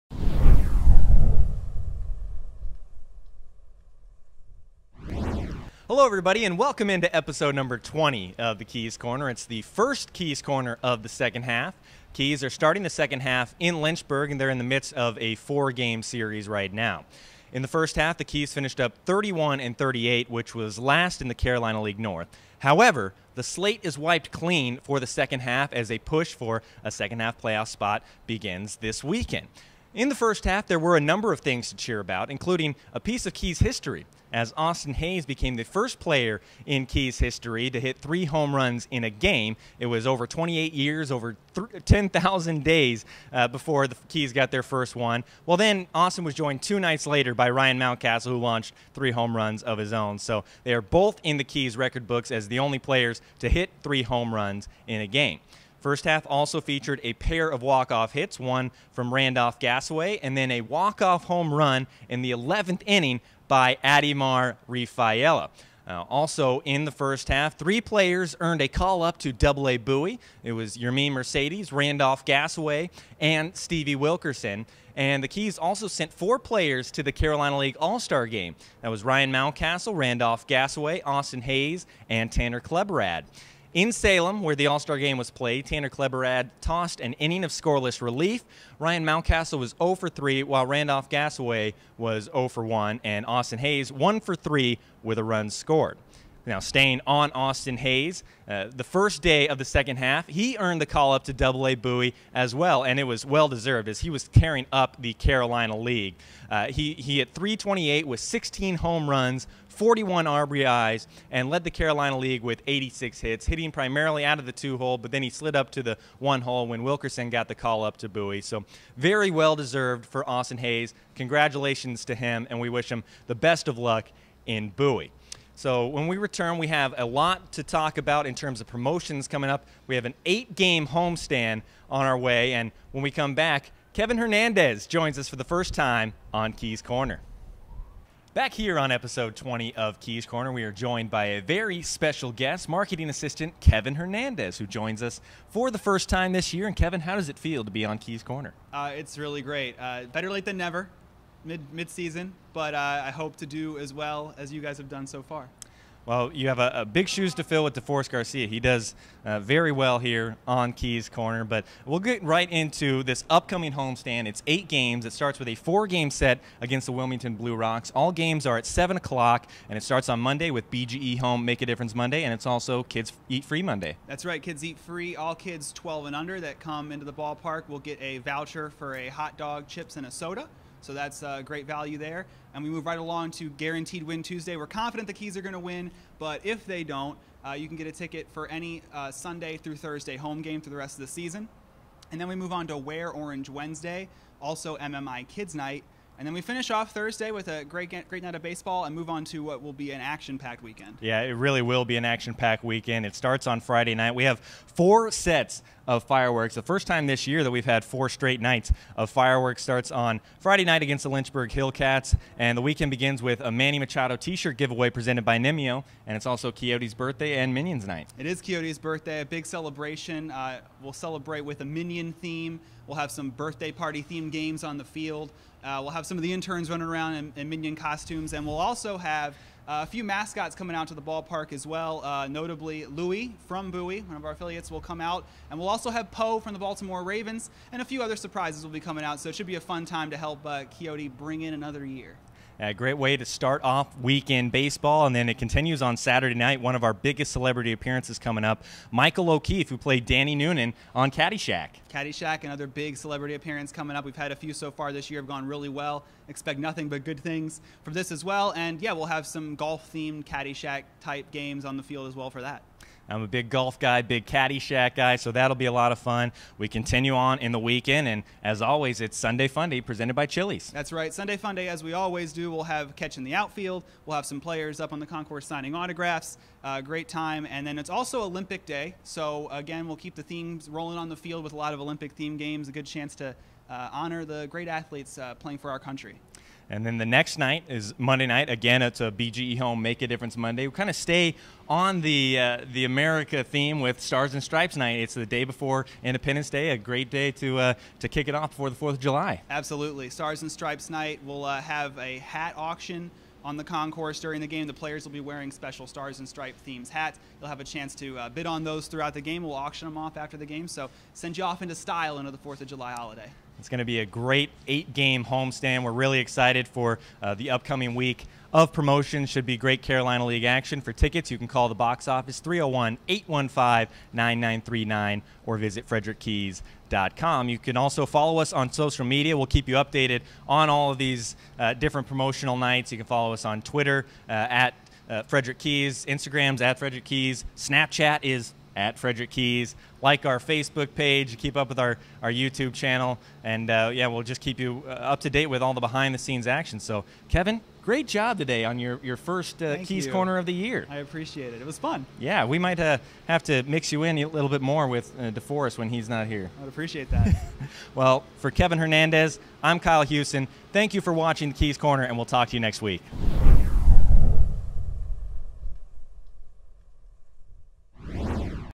Keys Corner - Season 2, Episode 20
@k_huse and special guest Kevin Hernandez recap the end of the first half and preview the promotions going on during the Keys 8 game home stand!